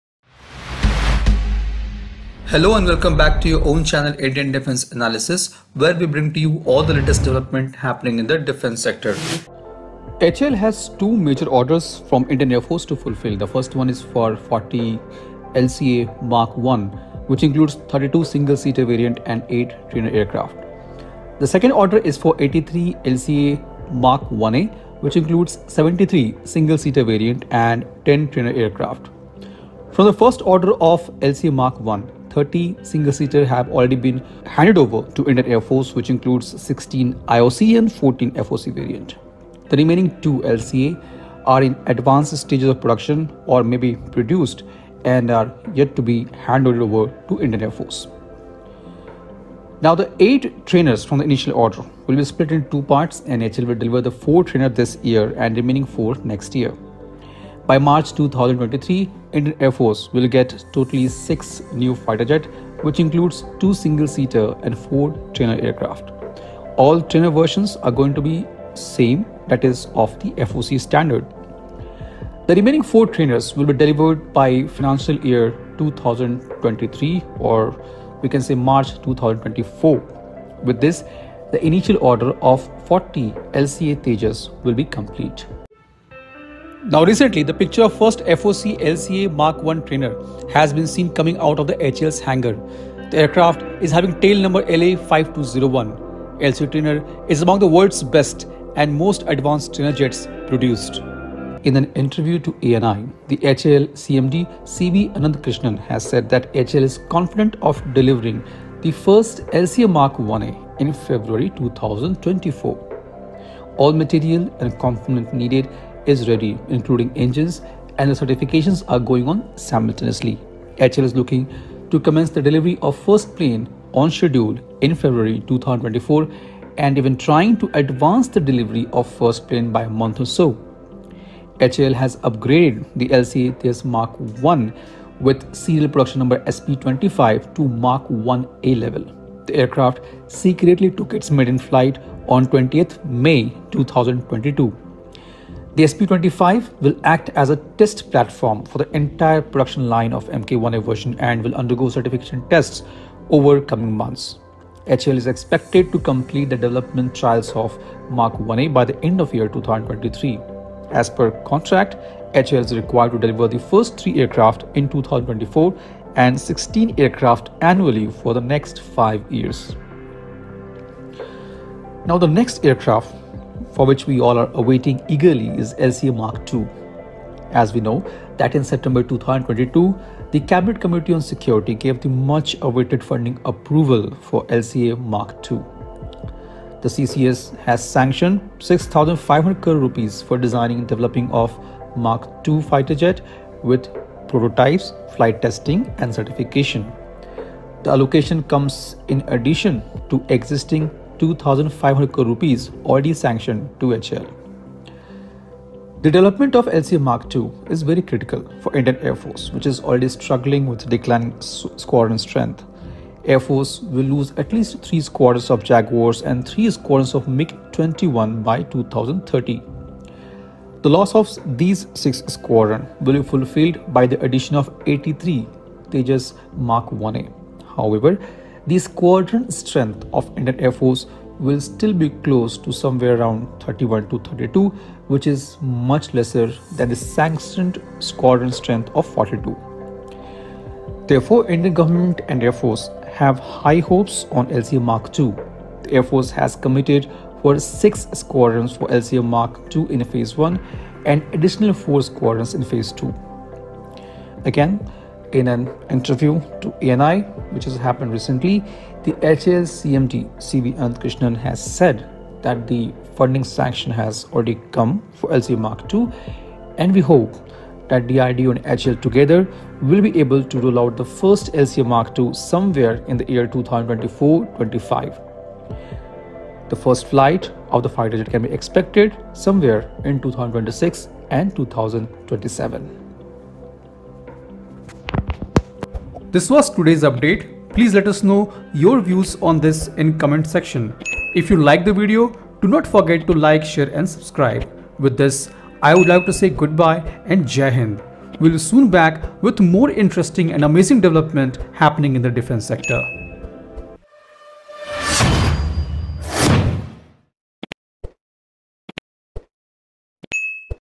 breakingnews 1st FOC LCA Mark1 trainer comes out of hanger
Light Combat Aircraft Orders

HAL has two major orders from Indian Air Force to fulfil. The first one is for 40 Tejas MK1 which includes 32 single seat variant and 8 trainer aircraft. The second order is for 83 Tejas Mark1A which includes 73 single seater variant and 10 trainer aircrafts.

From the 1st order of Tejas MK1, 32 Tejas single seater have been produced by HAL and handed over to Indian Air Force, which includes 16 IOC and 16 FOC variant.

The eight trainers from initial order will be split into two parts and HAL is expected to deliver 4 trainer in FY22 and remaining 4 in FY23. All the trainer versions are going to be the same and will be of FOC standard.

4 Trainer in FY23

The remaining 4 trainers will be delivered by the financial year 2023. With this the initial order of 40 LCA Tejas will be complete. In February 2023, the first picture of trainer version of LCA Trainer with tail number LT-5201 was shared. On 5th April,  a major milestone in the LCA Tejas Program was achieved when  the first ever series production standard LCA Trainer (LT 5201) manufactured by HAL took to the skies for its maiden flight today from HAL airport and landed after completing a successful sortie of around 35 minutes.

1st LCA Tejas Mark 1A in Feb 2024

In an interview to ANI, the HAL CMD CB Ananthakrishnan has said that HAL is confident of delivering the first LCA Mark1A in Feb 2024.

All the material and component needed is ready including engines and the certifications are going on simultaneously. HAL is looking commence the deliver the first plane on schedule in February 2024 and even trying to advance the delivery of the first plane by a month or so.

HAL has upgraded the LCA Tejas Mark1 with serial production number SP-25 to Mk1A level. The aircraft secretly took its first flight on 20th May 2022. The SP-25 will act as a test platform for the entire production line of the Mk1A version and will undergo certification tests over coming months. HAL is expected to complete the development trials of the Tejas MK1A by the end of year 2023. As per contract, HAL is required to deliver the first three aircraft in 2024 and 16 aircraft annually for the next five years.

LCA Mark II

In September 2022, the CCS gave the much-awaited funding approval for the LCA Mark-2. The CCS has sanctioned 6,500 crore rupees for designing and developing of Tejas Mark-2 fighter jet with prototypes, flight testing, and certification. The allocation comes in addition to the existing 2500 crore rupees sanctioned to HAL.

The development of LCA Mark-2 is very critical for Indian Air Force which is already struggling with the decline squadron strength. Indian Air force will lose at least 3 squadrons of Jaguars & 3 squadrons of Mig-21 by 2030. The loss of 6 Squadron will be fulfilled by the addition of 83 Tejas Mark-1A. However, the squadron strength of IAF will be still close to 31 or 32 which is much lesser than the sanctioned squadron strength of 42.

Therefore, Indian government and Indian air force has high hopes on LCA Mark-2. The air force has committed for 6 squadrons of LCA Mark-2 in Phase-I and additional 4 squadrons in Phase-II.

In an interview to ANI, the HAL CMD CB Ananthakrishnan has said that the funding sanction has already come for LCA Mark II. We hope DRDO and HAL together will be able to roll out the first LCA Mark II somewhere in the year 2024-25. The first flight of the fighter jet can be expected in 2026-27.